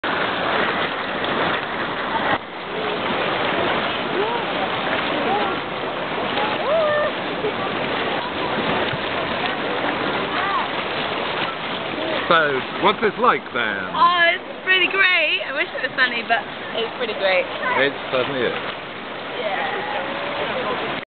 At the Baths in Budapest
We all (Tor, me , Lucy, Bridie, and Daisy) had a really relaxing time discoving the way of Hungarian Baths